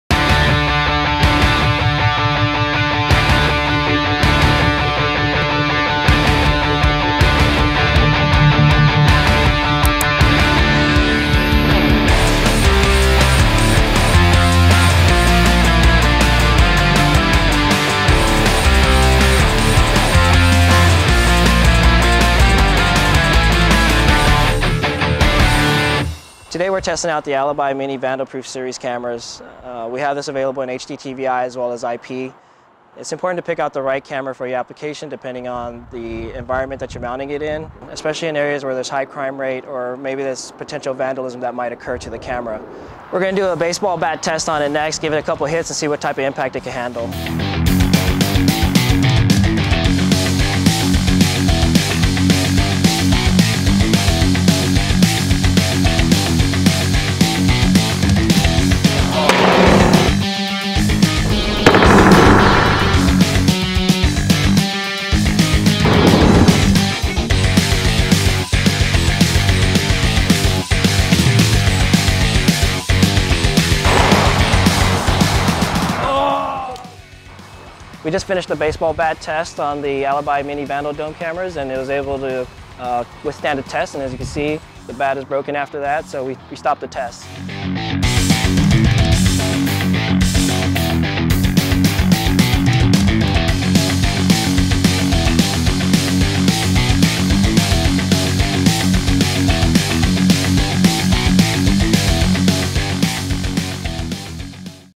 Vandalproof Dome Bat Test | IP 3.0 Megapixel 65 ft IR Security Camera
Testing the force of impact that this Alibi IP 3.0 Megapixel 4.0mm Fixed Lens 65 ft IR Vandalproof Dome Camera can handle by hitting it with a baseball bat.